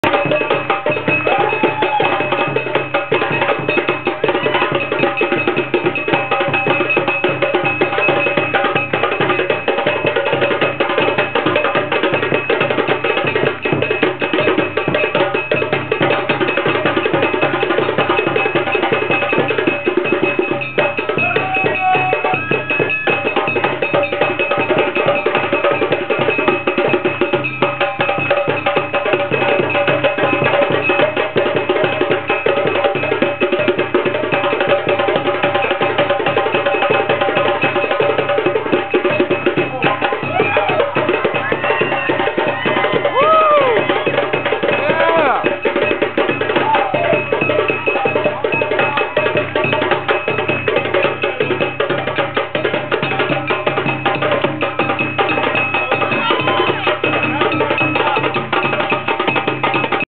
DUPONT CIRCLE FESTIVAL
Diversity is the word here. It is reflective of this lively neighborhood close to Georgetown University in our nation's capital. Ecclectic, colorful, awesome!